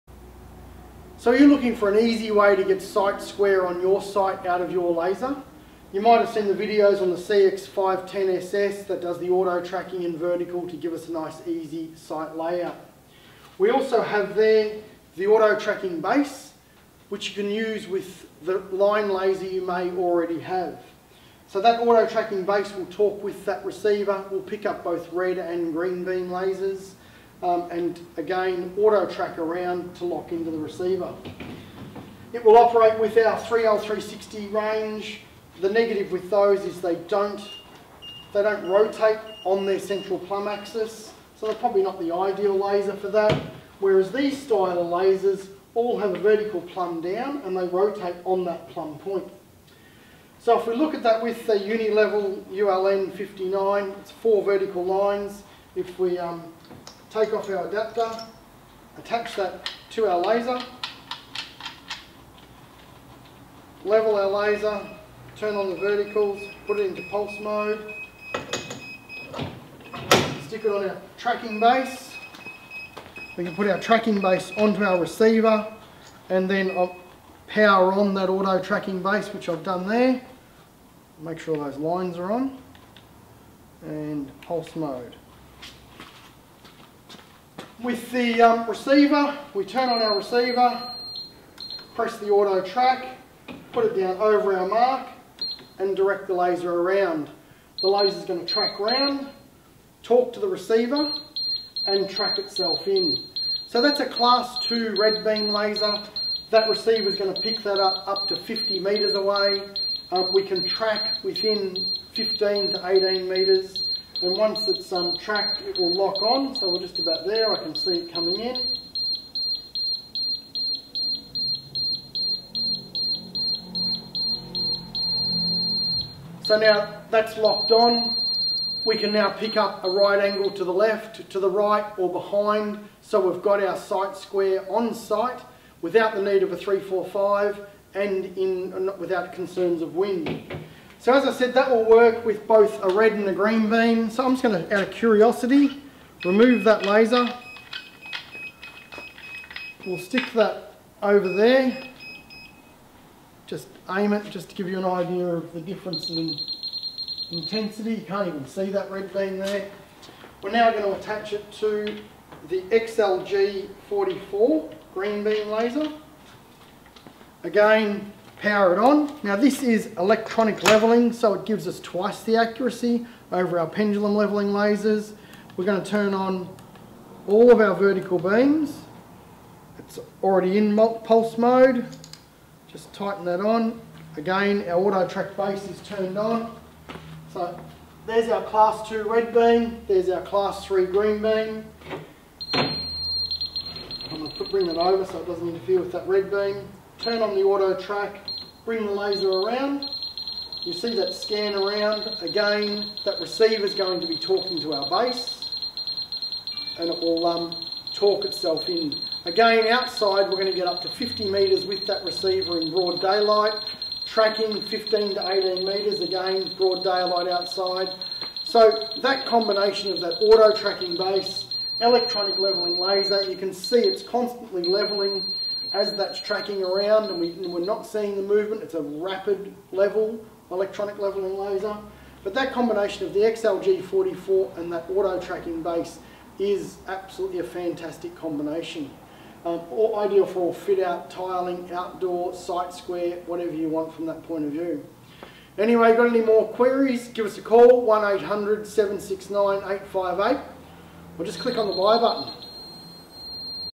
RedBack Lasers AUTO Track AT1 line laser level tracking base for site square layout and alignment
RedBack Laser Auto Tracking Base for most line lasers with pulse mode, either Red or Green.  The Auto Tract AT1 motorised base communicates with the included receiver to track a line lasers vertical beam to the receivers position.  This makes site alignment and site square layout a snap both indoor and outdoors.  You can check out the AT1 Auto Track

Video features the UN671D multi Line Laser and the RedBack XLG44 green multiline laser available